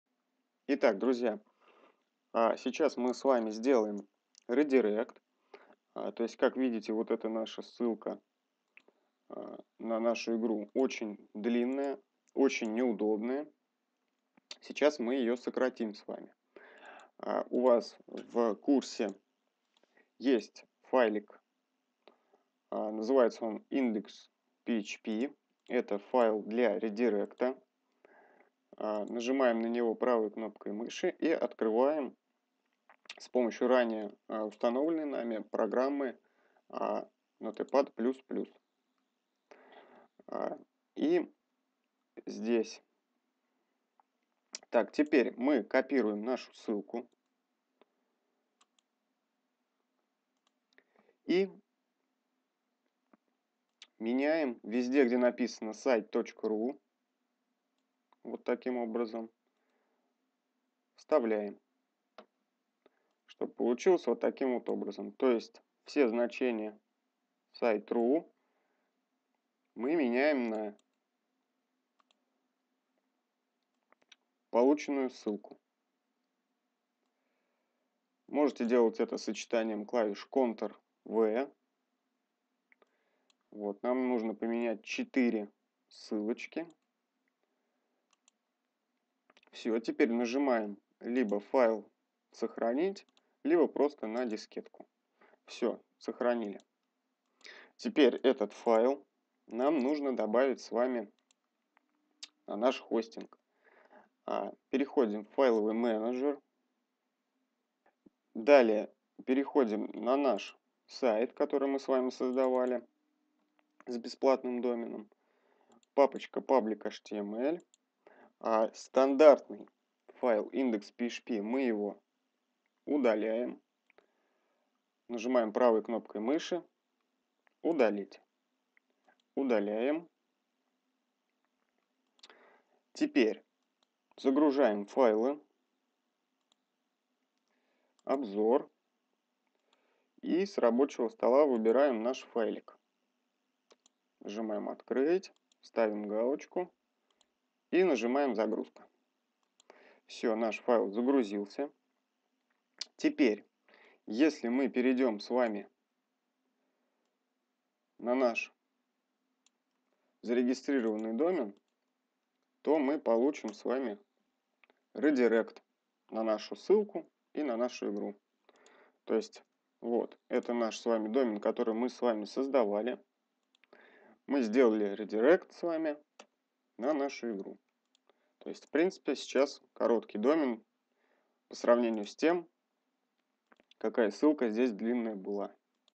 КАК СДЕЛАТЬ РЕДИРЕКТ САЙТА | СОЗДАНИЕ РЕДИРЕКТА Урок 8
Как сделать редирект на другой домен
КАК СДЕЛАТЬ РЕДИРЕКТ САЙТА | СОЗДАНИЕ РЕДИРЕКТА
👉 Помогите плз в развитии канала!
Также пишите, что вам интересного заснять)